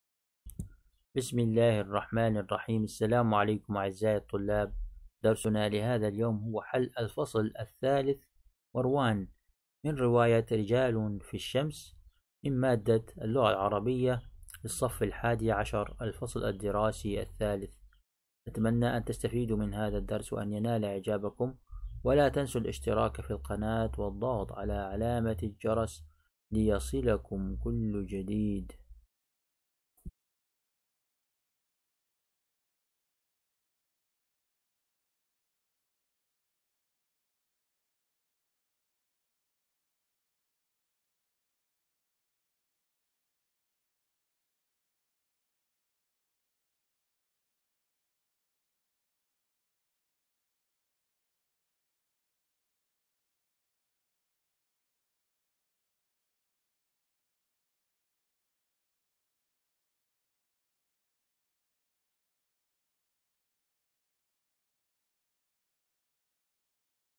حل درس ( مروان ) مع التلخيص رواية رجال في الشمس - عربي حادي عشر
حل رواية رجال في الشمس
اللغة العربية للصف الحادي عشر
غسان كنفاني